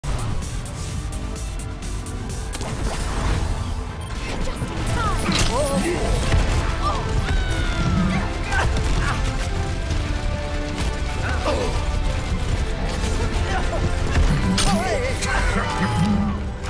Boop 2 - The Boopening. Highlight
Not much to say, it was the second time I harassed this roadhog with boops off the edge, and this time he actually was sad about it.